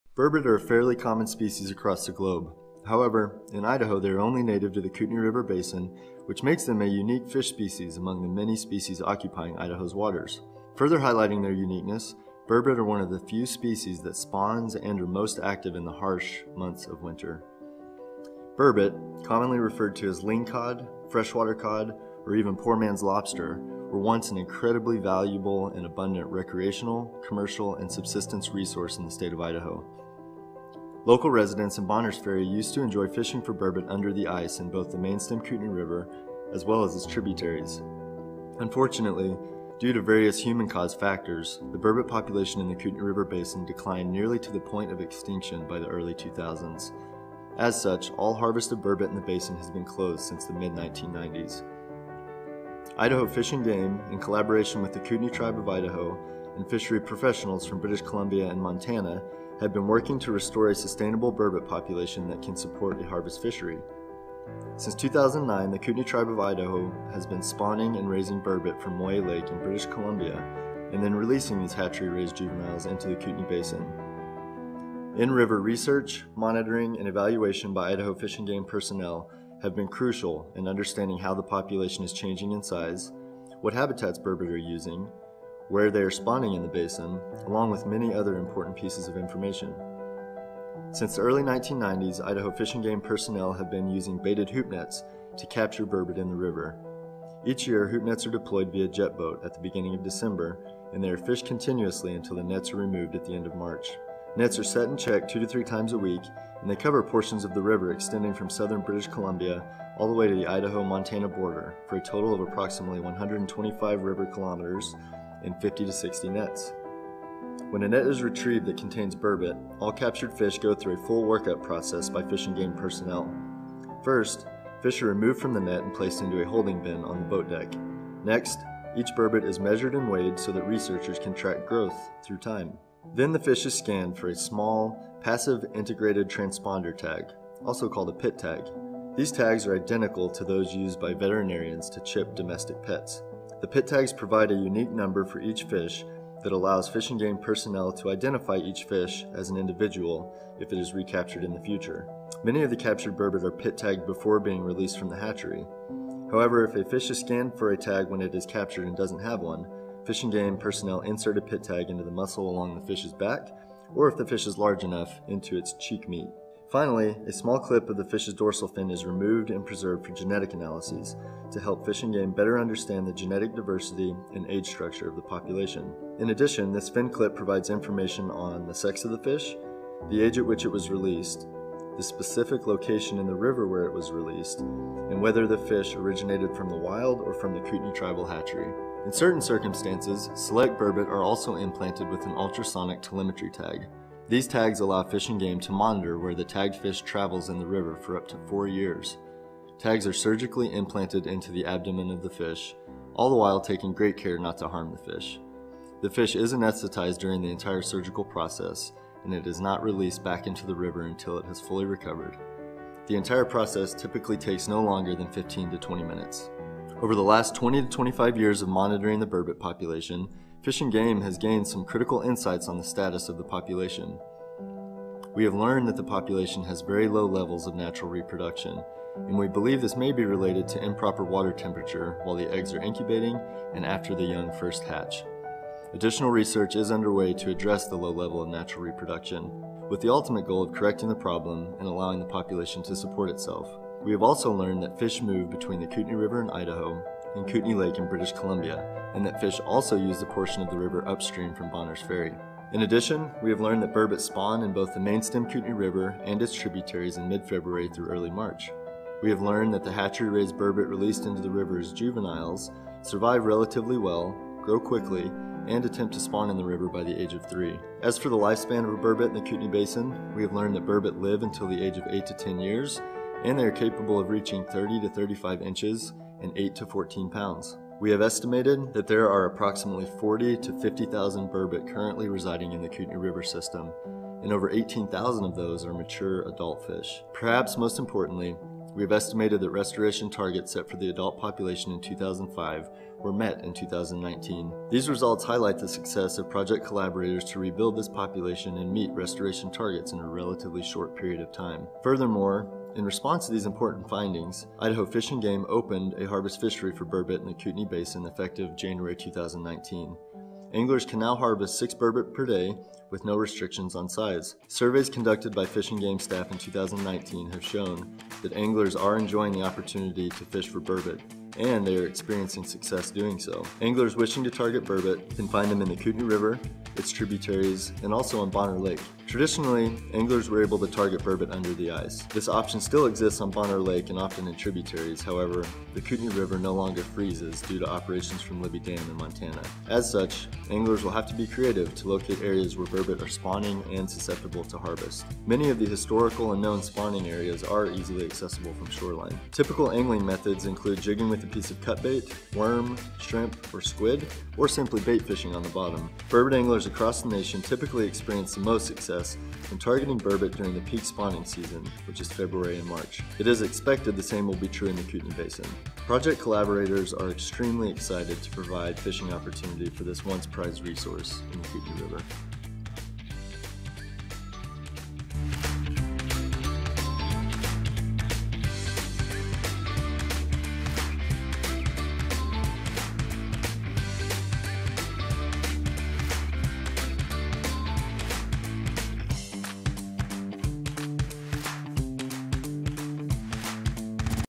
The decline and recovery of burbot in the Kootenai River.
Burbot once thrived in the Kootenai River in North Idaho. A combination of activities and events nearly wiped them out. After decades of research, experimentation and collaboration the burbot are back and fishing is on.  AB/TJR  2019